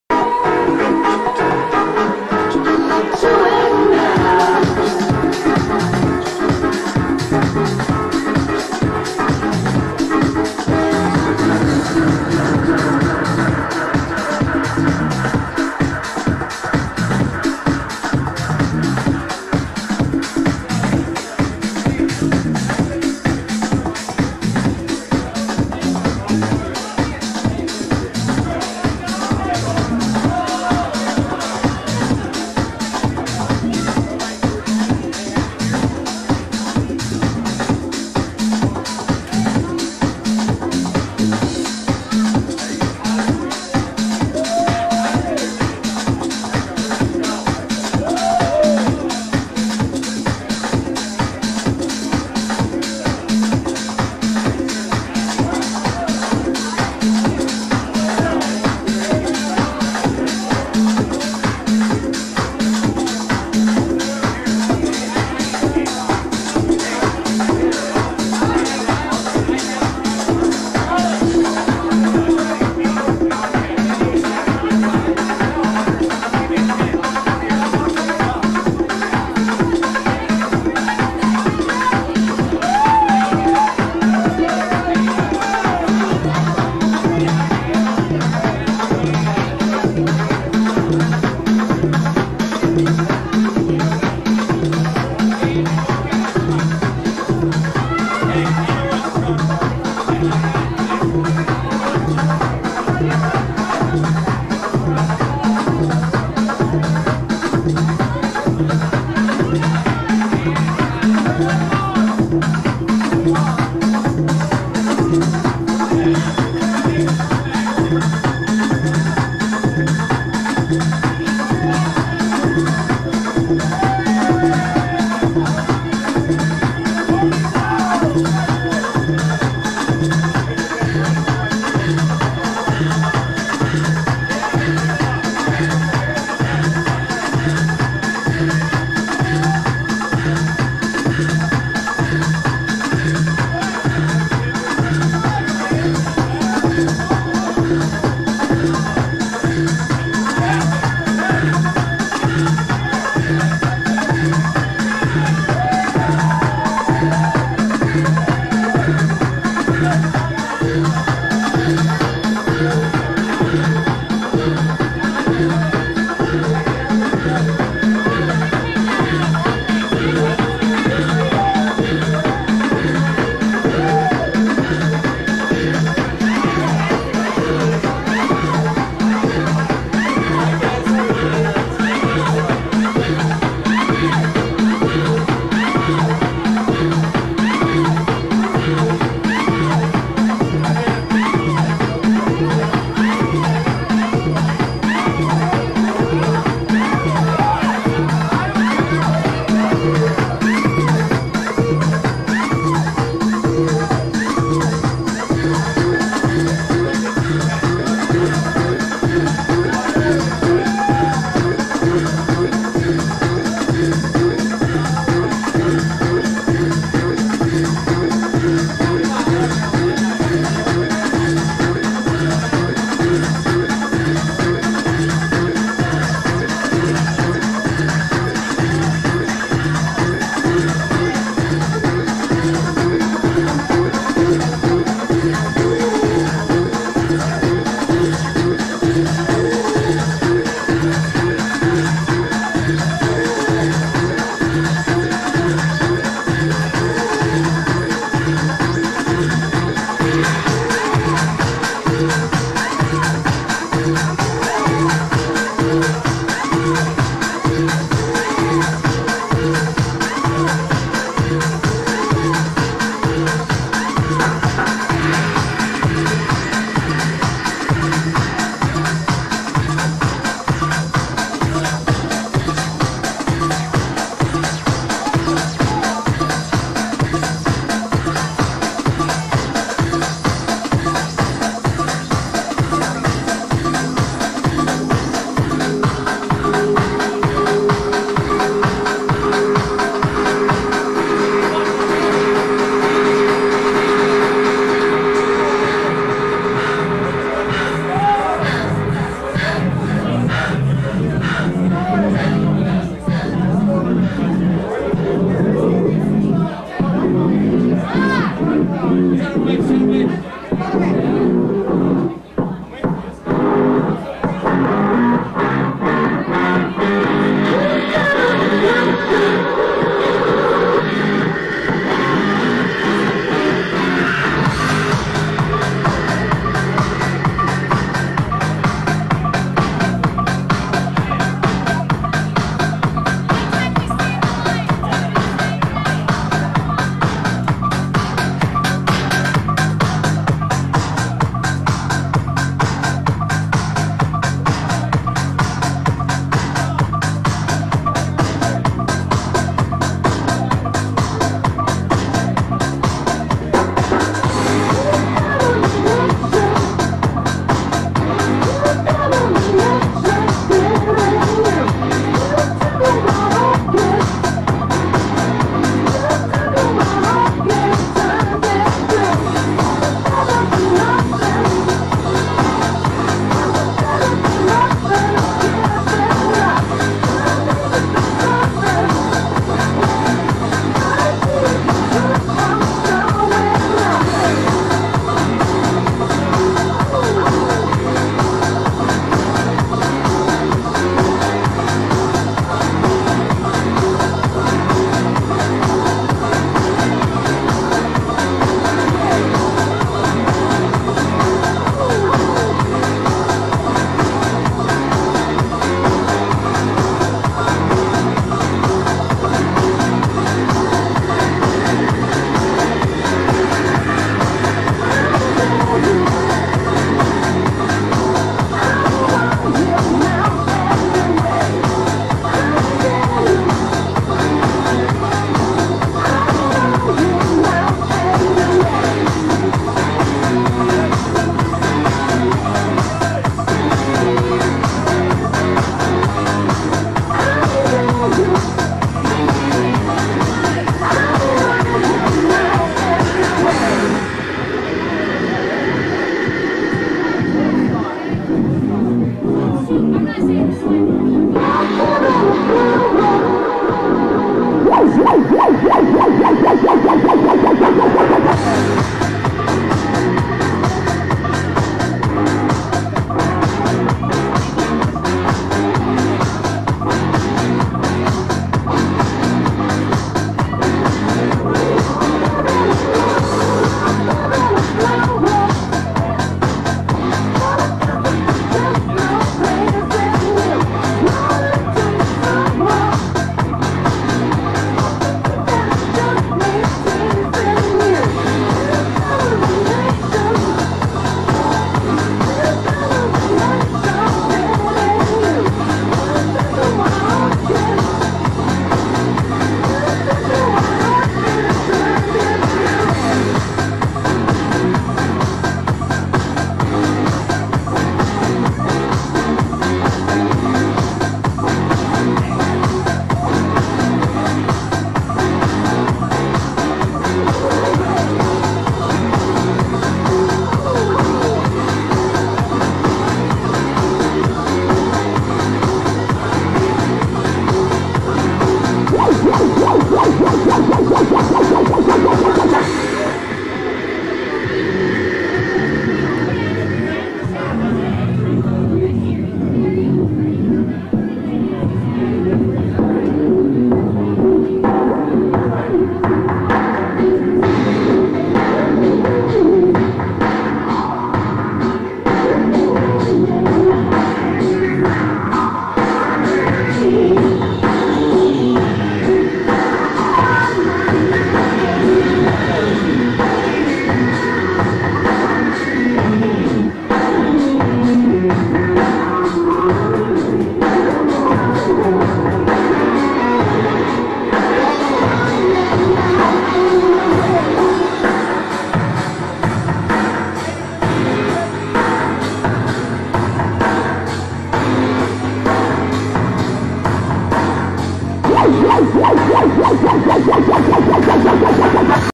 515Alive 3.1
Iowa's Urban Music and Art Festival
held on July 29th 2006 in a four block area of downtown Des Moines. There were a total of nine stages and 72 performers, along with BMX & Skate demos, breakdancers, and graffiti artists. It was an all ages free event.
3.1 features Zero of Dziner Muzik